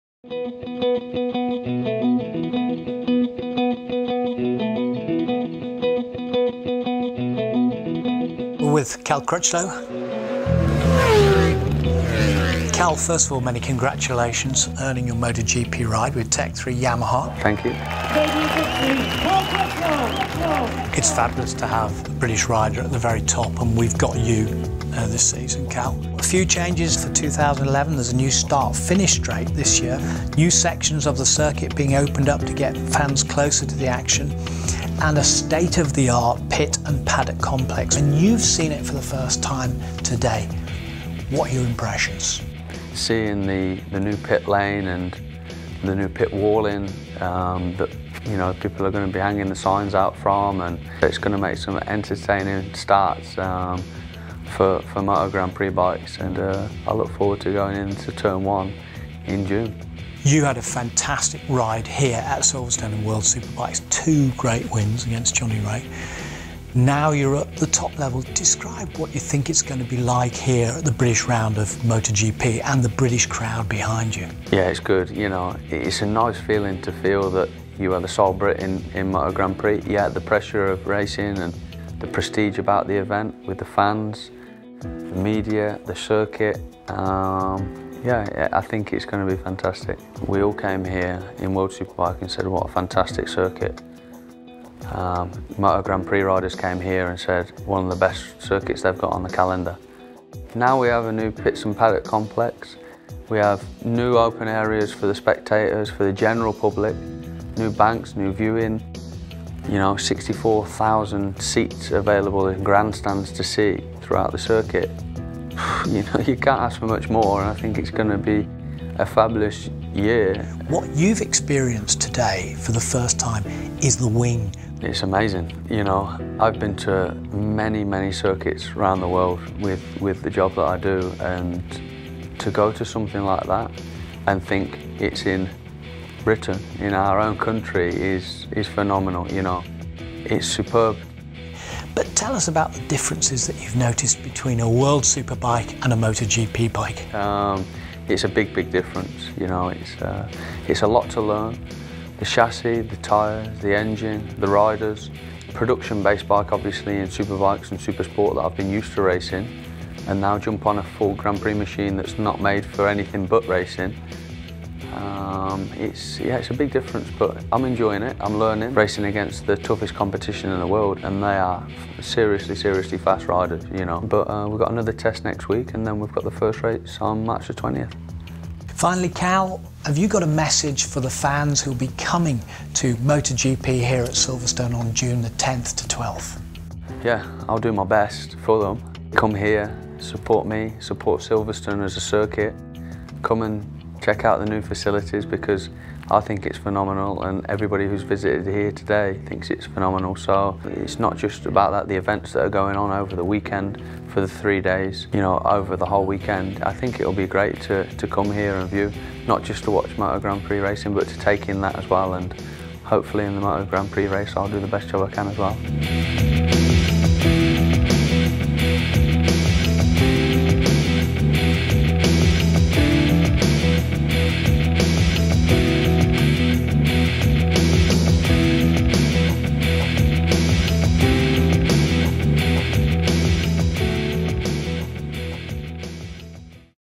Cal Crutchlow chats MotoGP, the British Grand Prix and checks out The Silverstone Wing
British MotoGP rider Cal Crutchlow chats MotoGP, the British Grand Prix (MotoGP), and checks out Silverstone's state-of-the-art new Pit and Paddock Complex, which will be used at the 2011 AirAsia British Grand Prix on 10-12 June